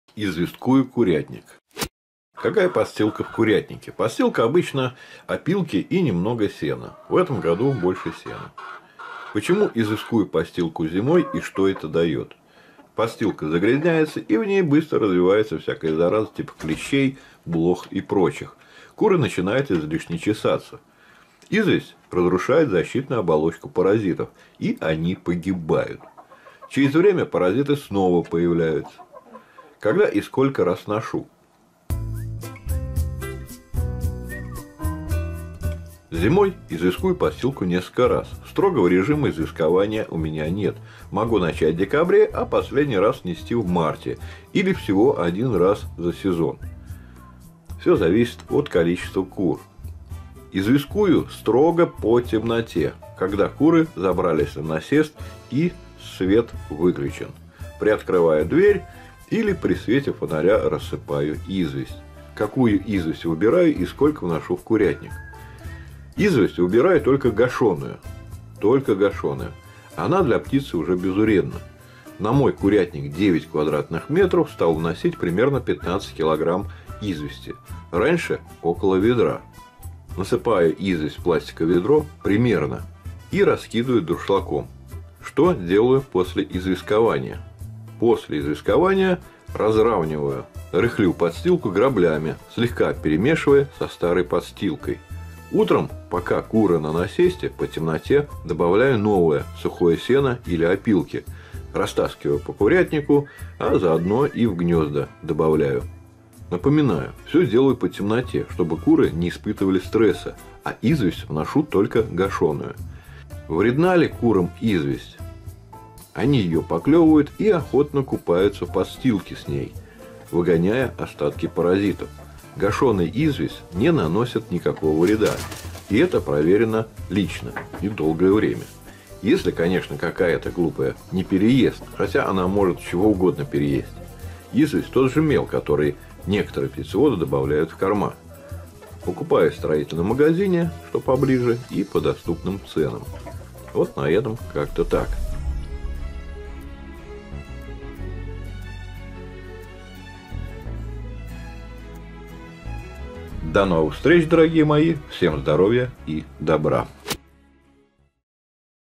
Известкую подстилку в курятнике с курами зимой. От паразитов
Известкую подстилку (стружка, опилки + сено) в курятнике с курами зимой.
С Вами Вячеслав
Помочь каналу очень просто:
Поставь 👍 или 👎 видеоролику
Напиши комментарий под видео